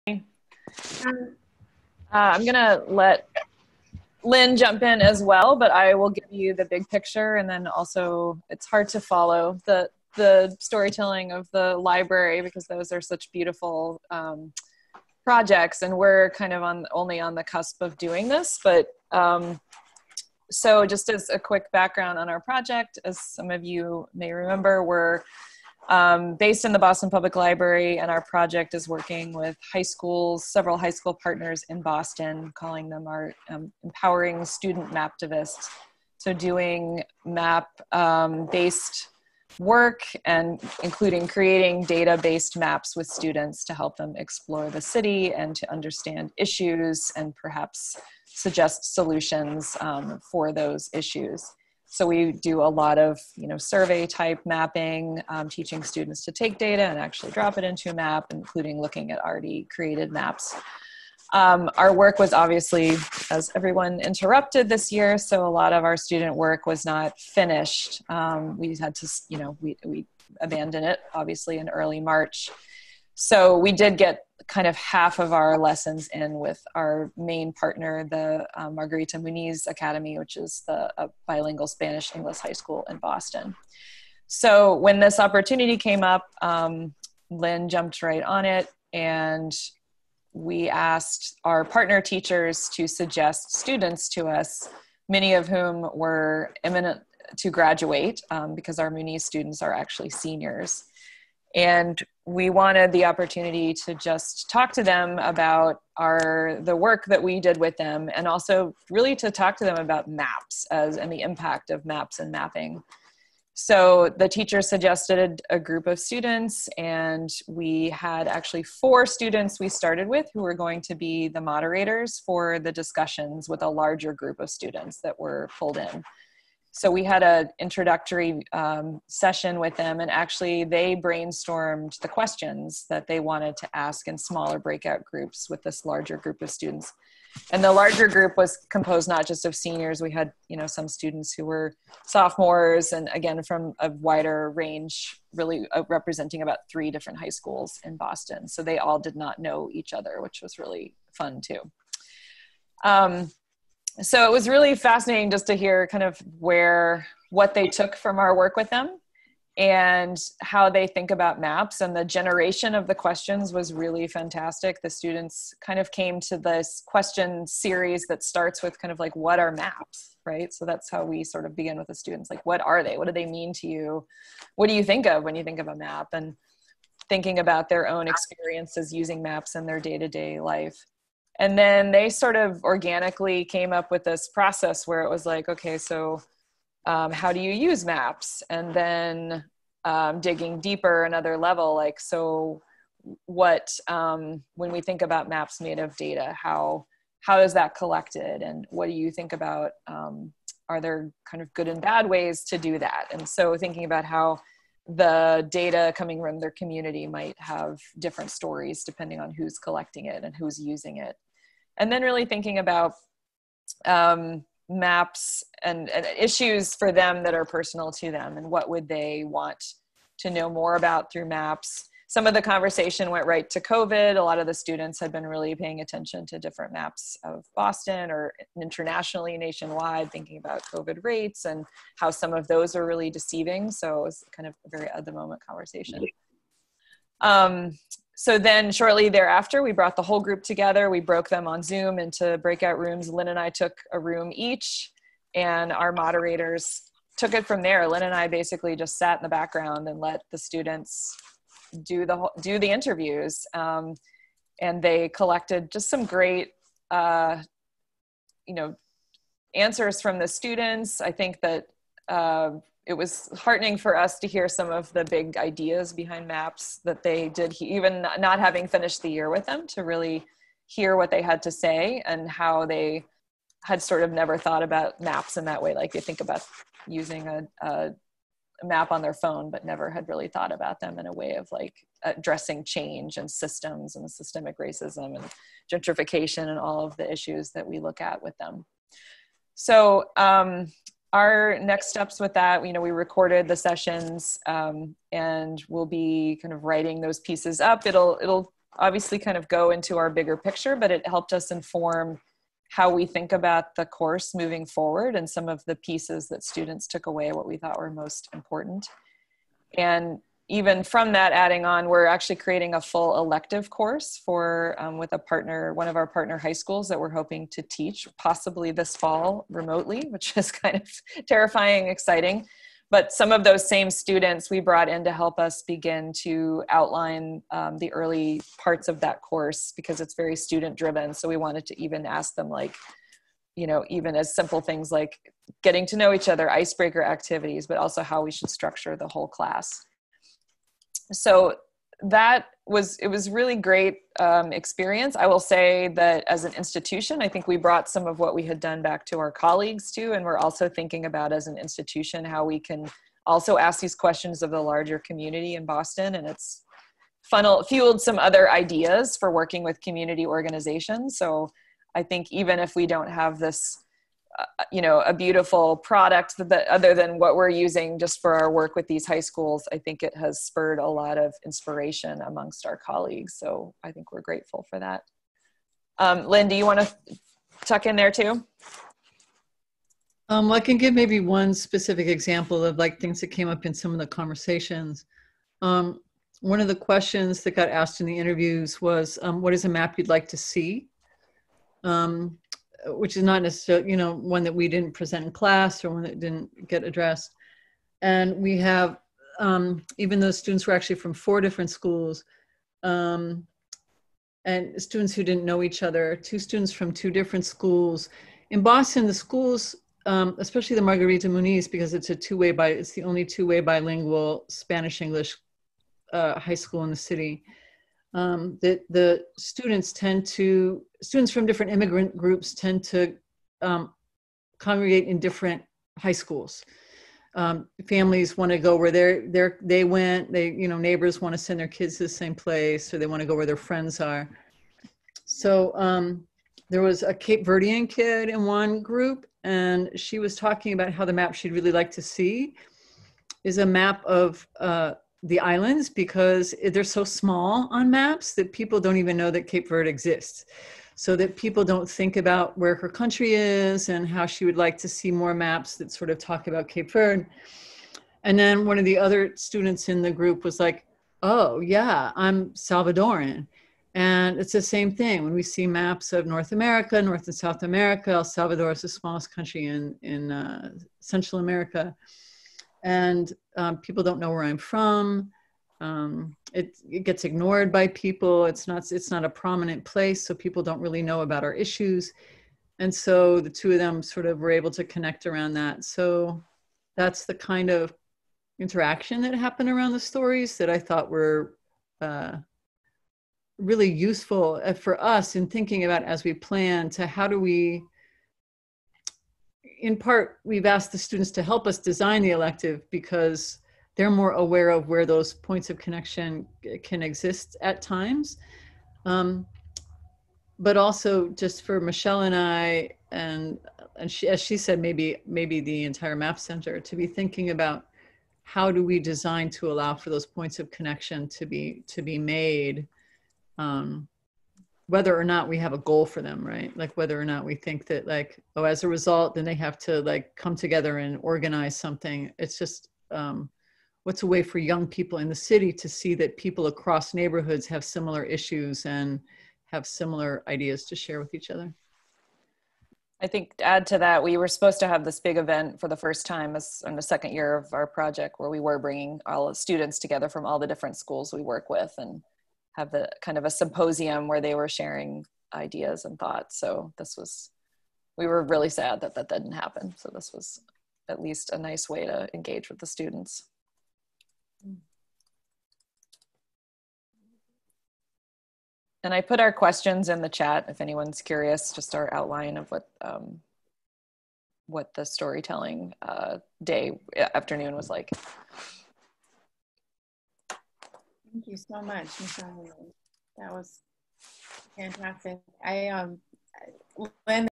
Leventhal Map Center (Boston) - Youth-Led Storytelling Sparks New Approach
Leventhal Map Center shares how they engaged young people who took part in the IMLS-funded “Empowering Maptivists” program to design and lead a storytelling process. In addition to surfacing powerful stories and building bonds between students, this experiment sparked new energy in their institution to support youth leadership and prioritize relationship-building as a goal in itself.

This presentation was created as part of the Community Catalyst Initiative funded by the Institute of Museum and Library Services (IMLS).

For information on...